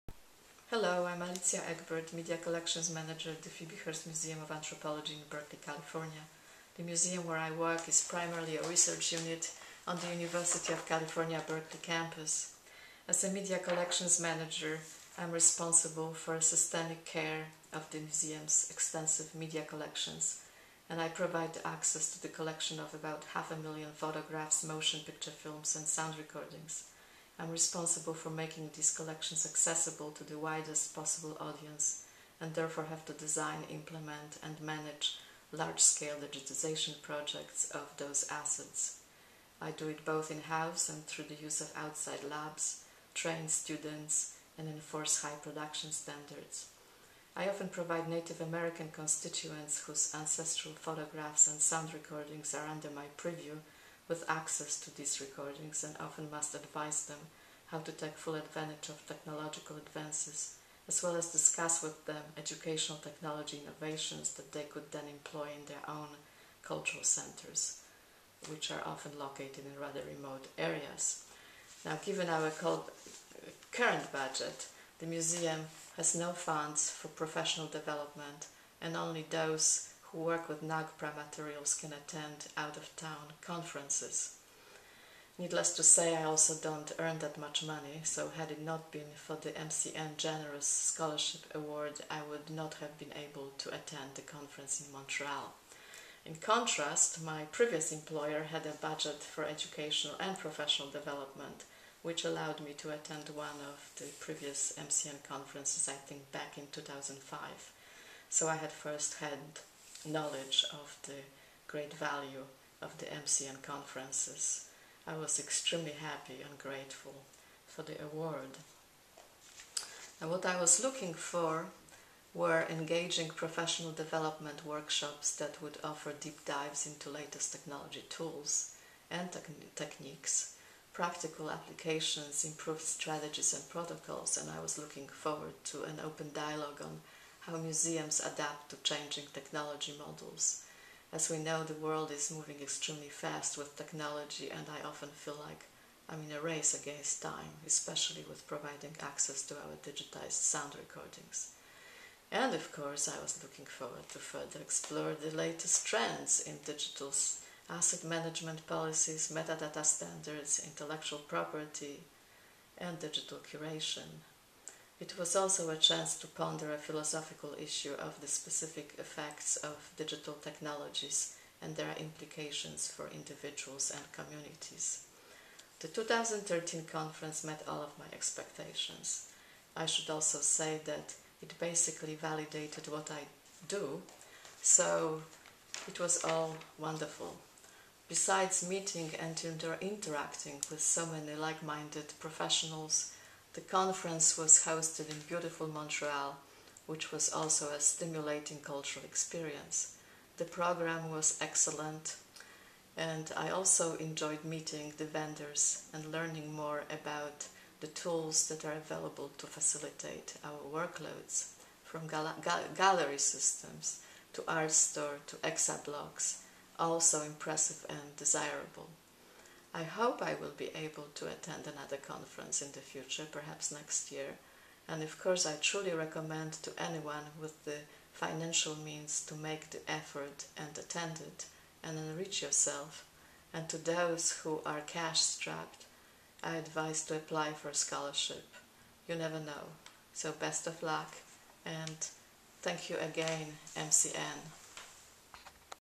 MCN Scholarship Testimonial: Alicja Egbert
MCN 2013 scholarship recipient Alicja Egbert shares her impressions of the MCN2013 conference in Montreal, Canada.